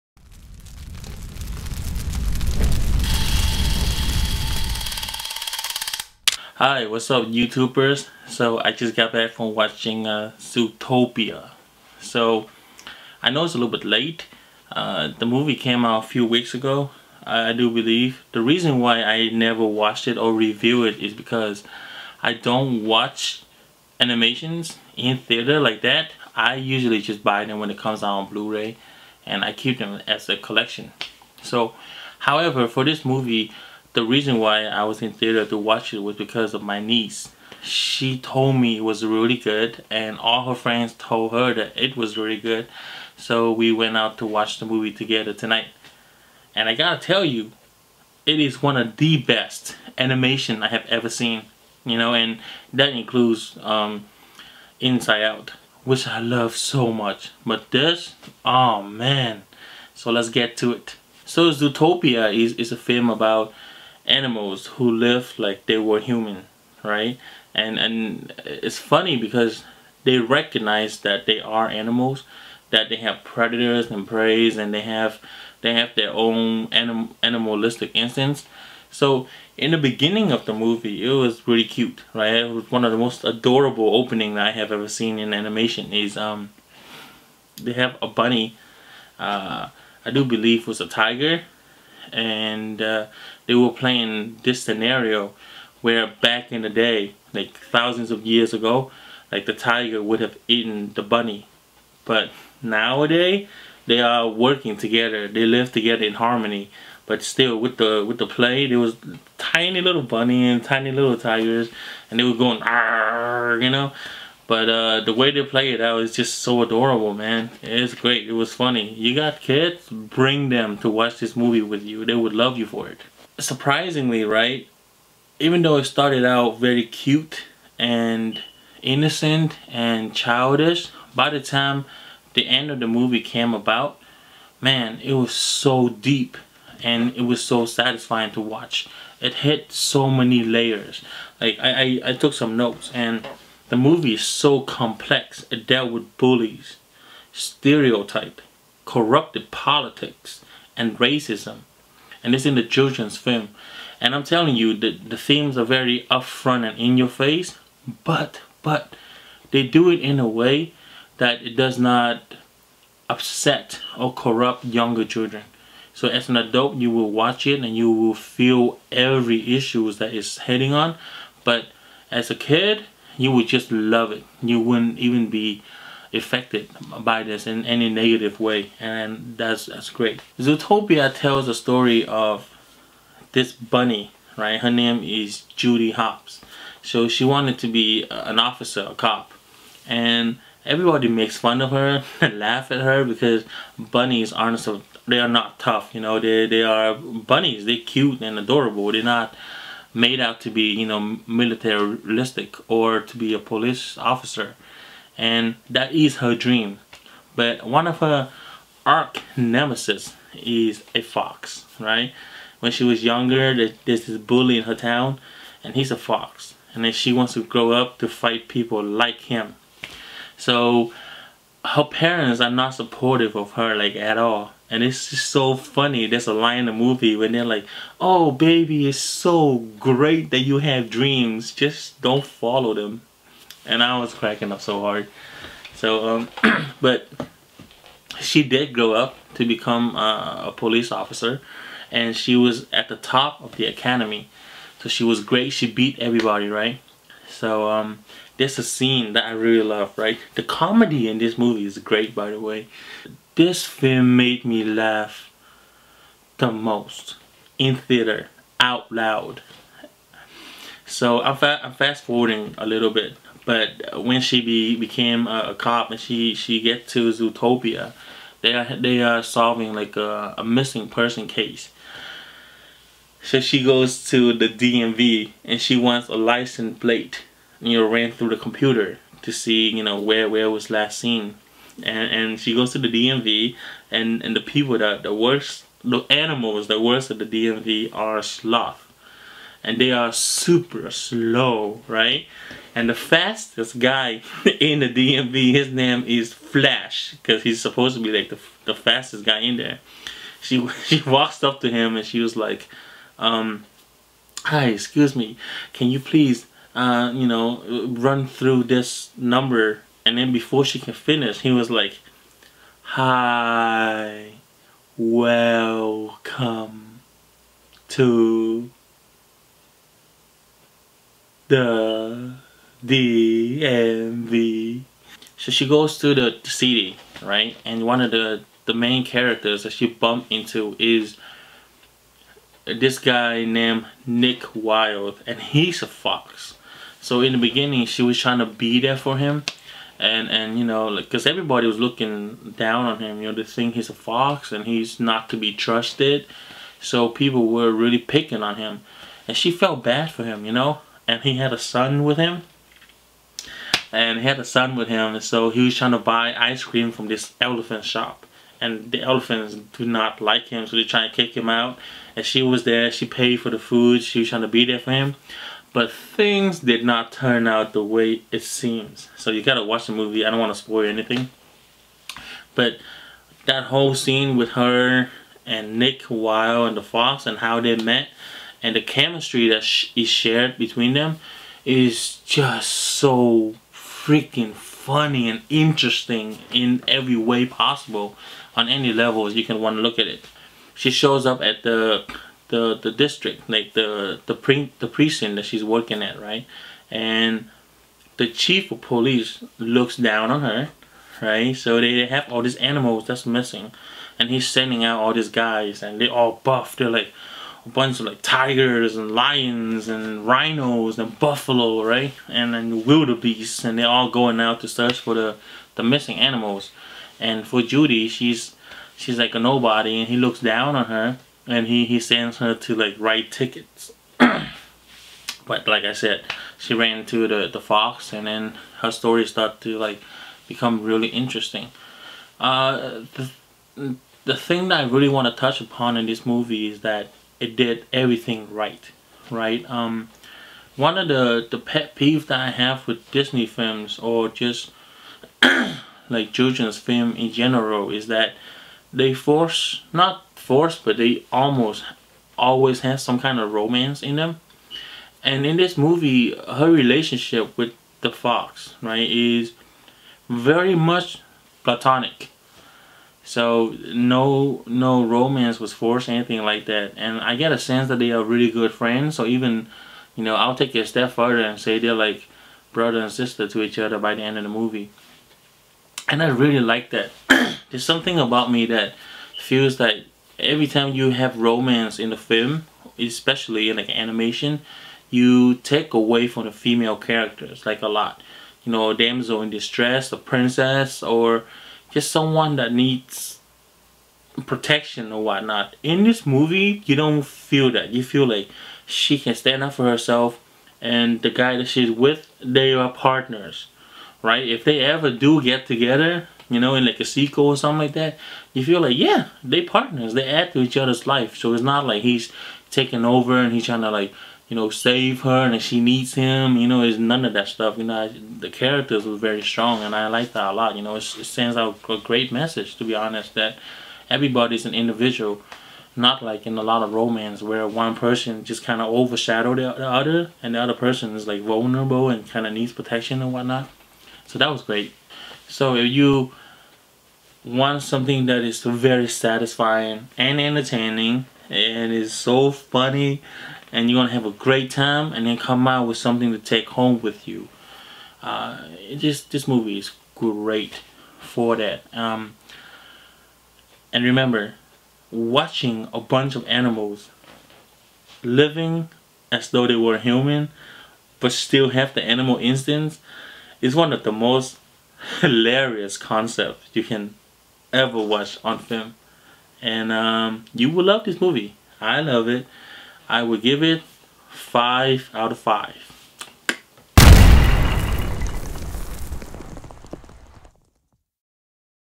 ZOOTOPIA REVIEW
A bunny with an impossible dream of becoming an officer in a city where tigers, lions and wildebeests runs the police department.  Teaming up a a sly fox, the bunny discover her true talent and saves the city of Zootopia.